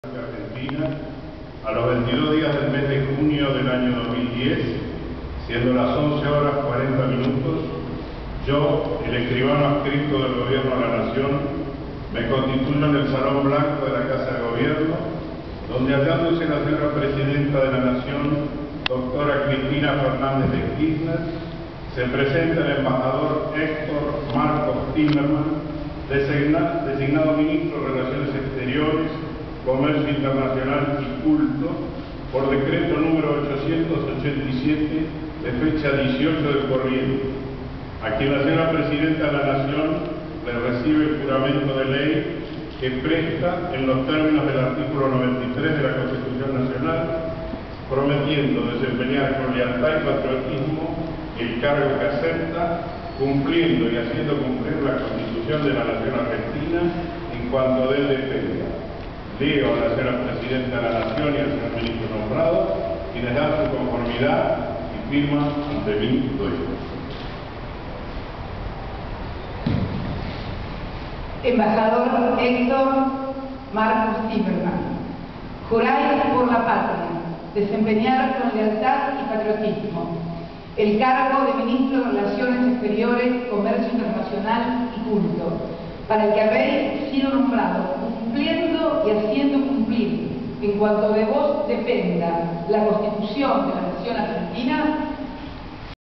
Timerman momento de la jura
Timerman asumió la conducción del Ministerio de Relaciones Exteriores y Culto en un acto que se realizó en el Salón Blanco de la Casa Rosada